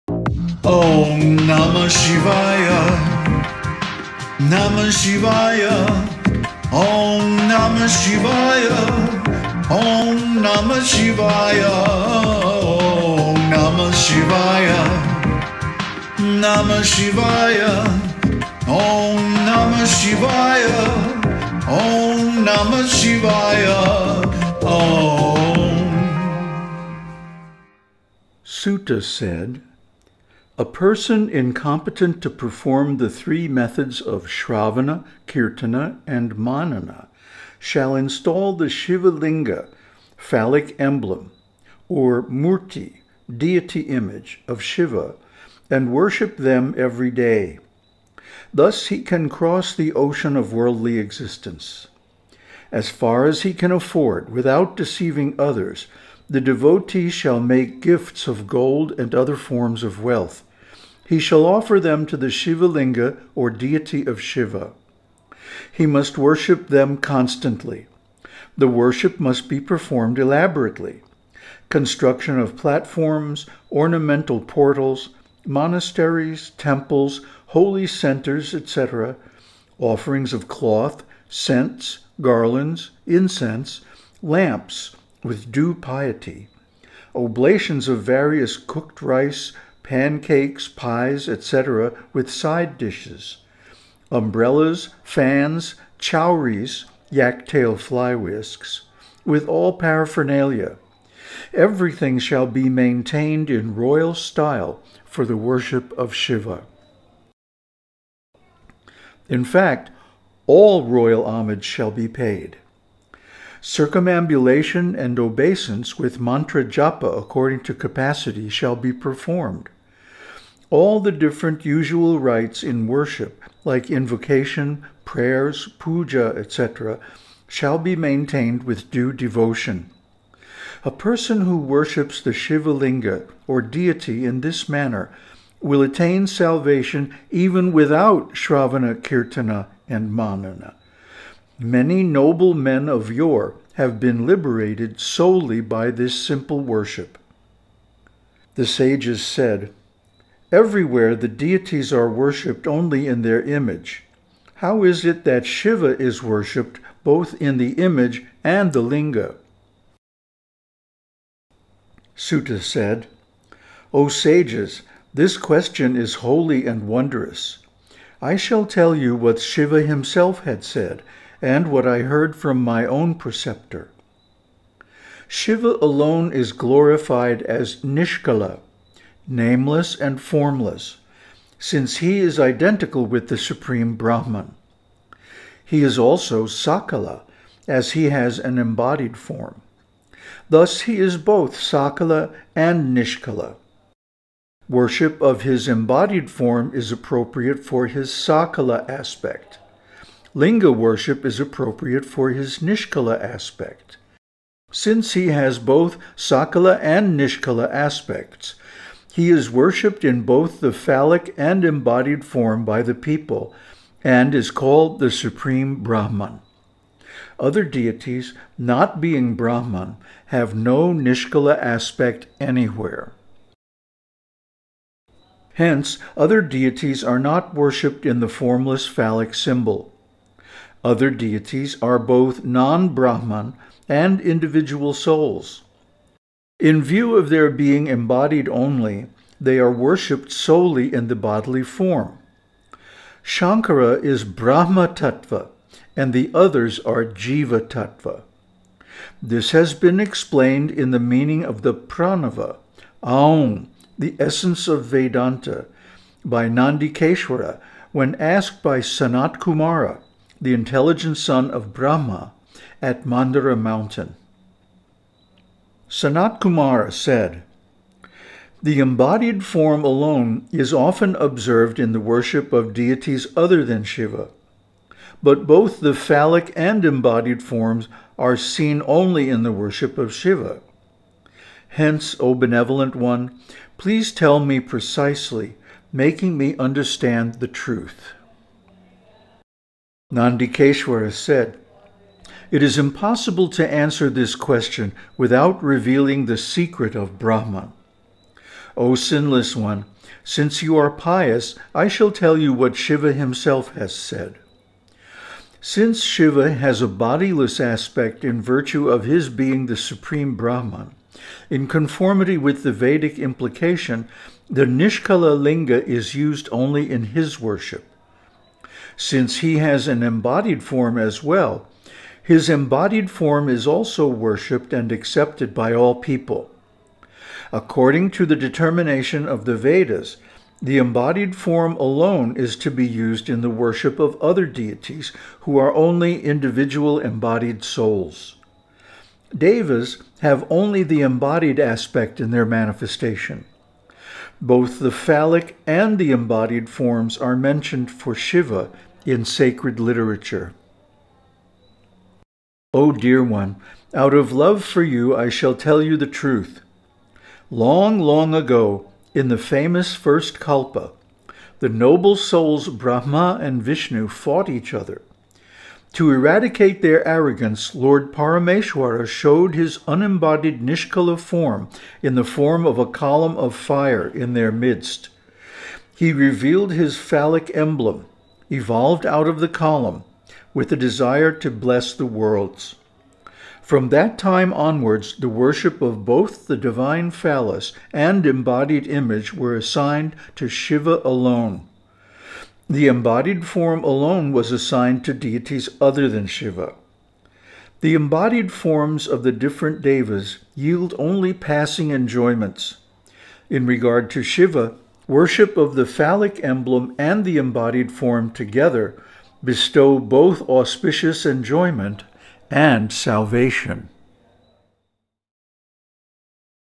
Śiva Purāṇa Vidyeśvara-saṁhitā 5—Greatness of Śiva-liṅga
Sūta said, “A person incompetent to perform the three methods of śravana, kīrtana and manana shall install the śiva-liṅga (phallic emblem) or mūrti (Deity image) of Śiva and worship Them every day. Thus he can cross the ocean of worldly existence. Since Śiva has a bodiless aspect in virtue of His being the Supreme Brahman, in conformity with the Vedic implication, the niṣkala-liṅga is used only in His worship. Since He has an embodied form as well, His embodied form is also worshipped and accepted by all people. According to the determination of the Vedas, the embodied form alone is to be used in the worship of other deities who are only individual embodied souls. Devas have only the embodied aspect in their manifestation. Both the phallic and the embodied forms are mentioned for Śiva in sacred literature.”